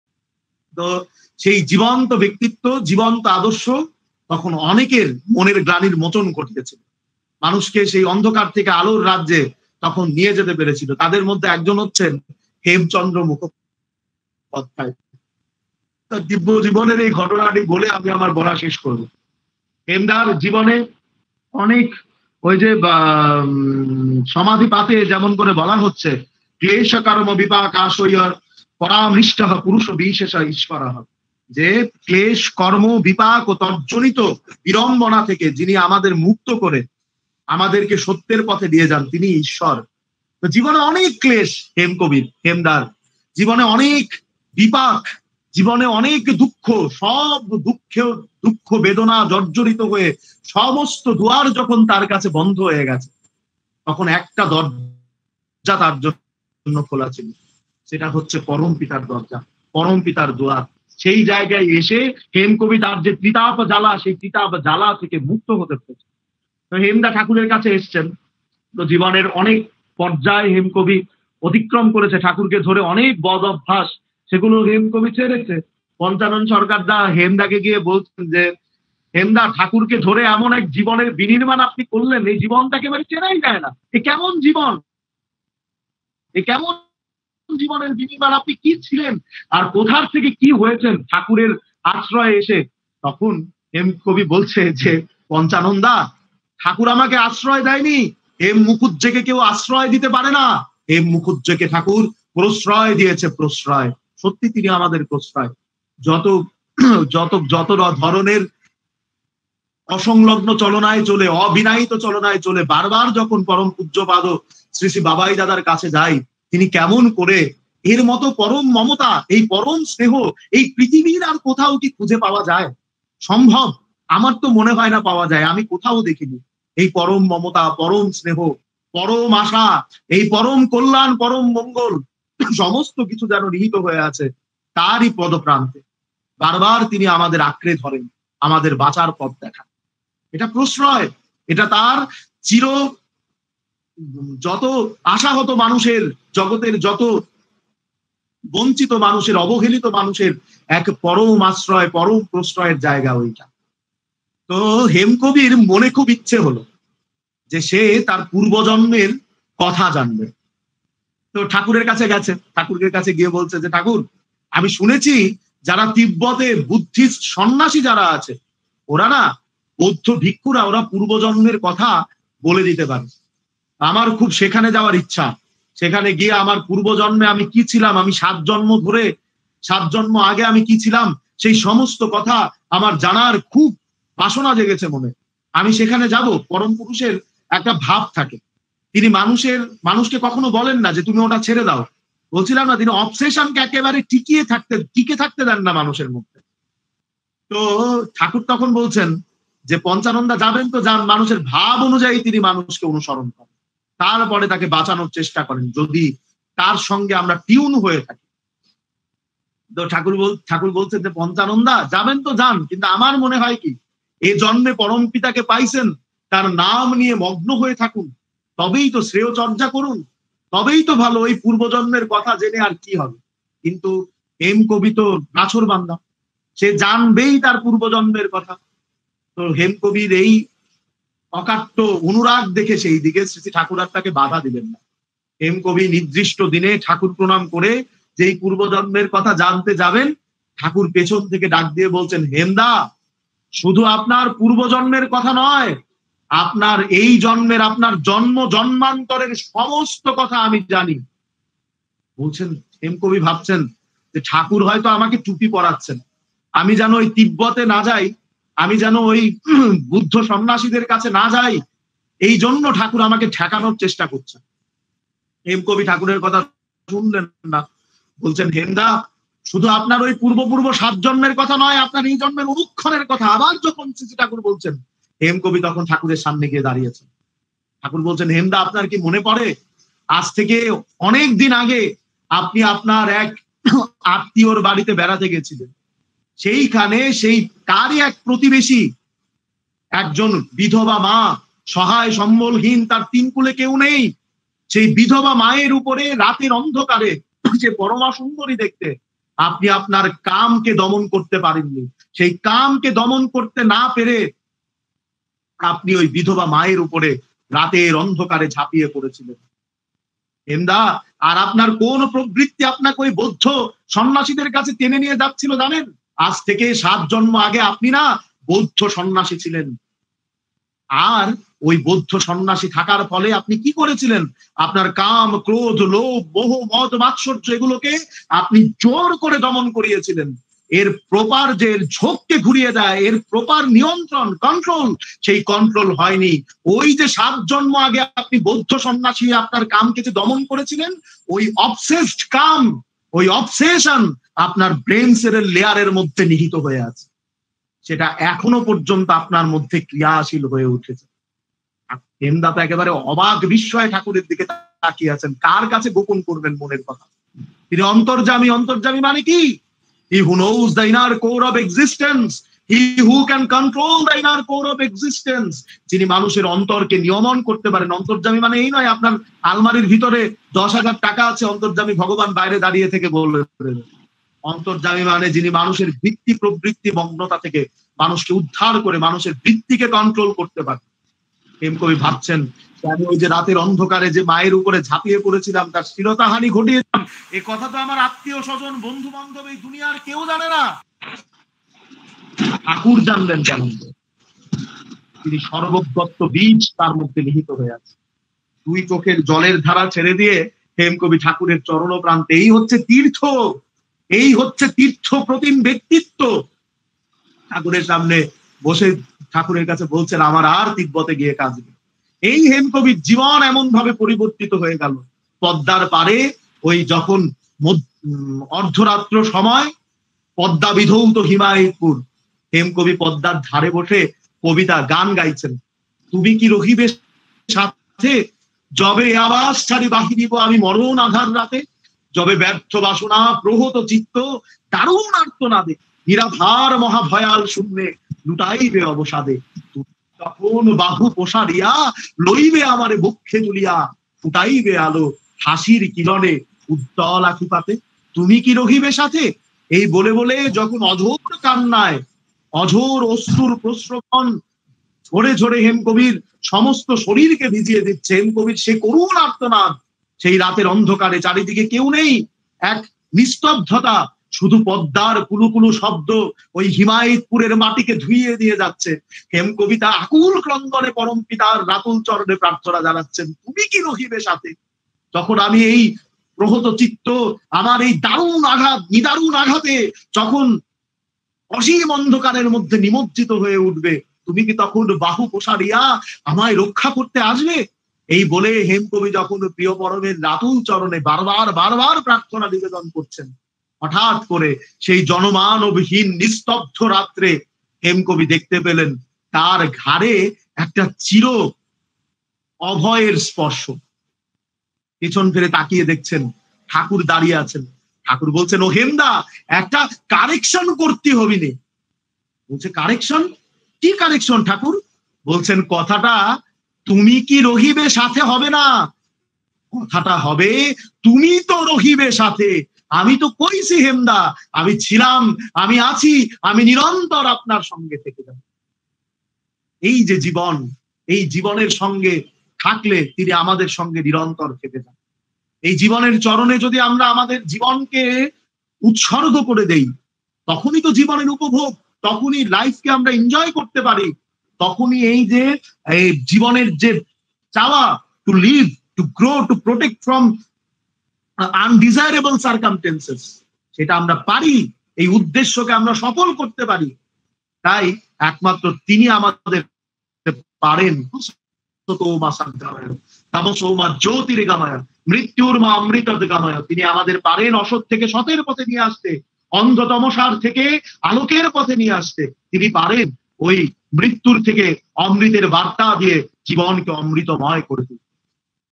ইষ্টপ্রসঙ্গ । শ্রীশ্রী ঠাকুরের কৃপায় হেমকবির জীবনীয় উত্থানের কাহিনী । আলোচনায়- ডাঃ সায়ন্তন চক্রবর্তী
A real life incident of Hemchandra Mukherjee (aka. HEMKOBI) according to Sri Sri Thakur Anukulchandra's Ideology by DR. Sayantan Chakravarty from Bangladesh in a Web Satsang organized by  on 8th August, 2021.

🙏🙏 Joyguru 🙏🙏🙏